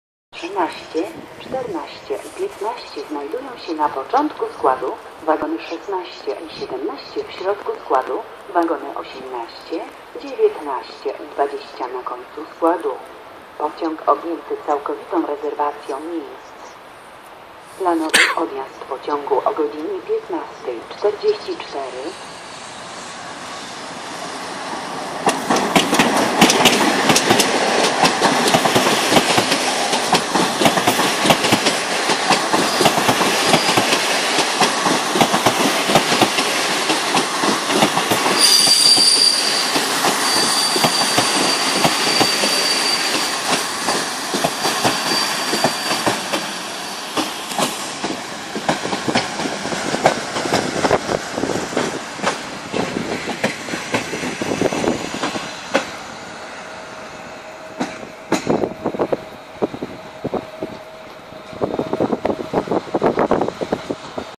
EP09-014 z TLK 31104 Janusz Korczak wjeżdża na stację Warszawa Zachodnia.
EP09-014 z TLK 31104 Janusz Korczak relacji: Kraków Płaszów - Białystok wjeżdża na stację Warszawa Zachodnia.
Data nagrywania filmu: 29 marca 2013.